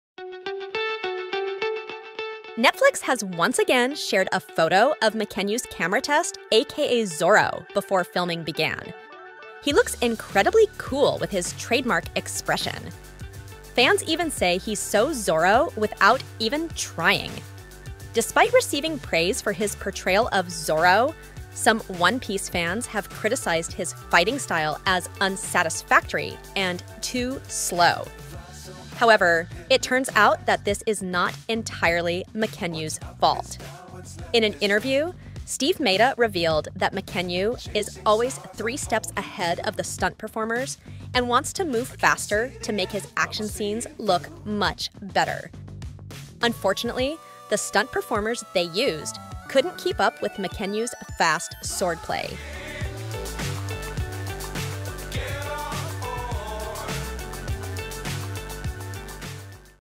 So, this is why Zoro's fight scenes in One Piece LA are slower than that of Enishi in Kenshin..
Netflix has once again shared a photo of Mackenyu's camera test a.k.a Zoro before filming began. He looks incredibly cool with his trademark expression. Fans even say he's so Zoro without even trying.

Despite receiving praise for his portrayal of Zoro, some One Piece fans have criticized his fighting style as unsatisfactory and too slow. However, it turns out that this is not entirely Mackenyu's fault.

In an interview, Steve Maeda revealed that Mackenyu is always three steps ahead of the stunt performers and wants to move faster to make his action scenes look much better. Unfortunately, the stunt performers they used, couldn't keep up with Mackenyu's fast swordplay.